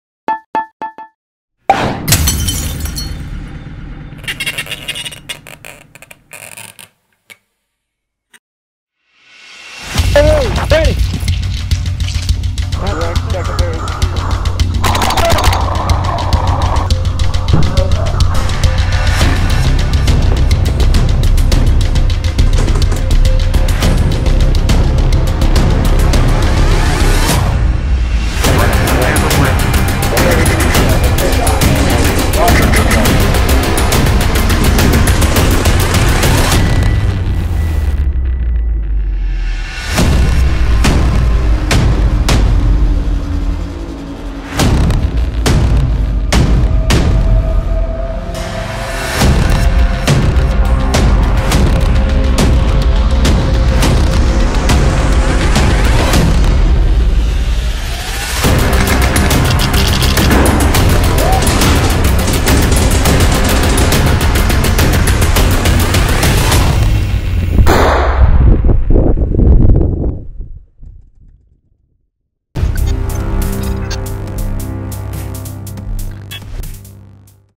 Eclipse HDE Urban Camo
Our HDE range of camo gear has been helping players hide in the woods – with style – for many years and now it's time to help those who fight their battles in urban landscapes.

Loader Fed, Mag Fed, Airsoft... whatever your field of play our HDE camo blends right in.

VID0129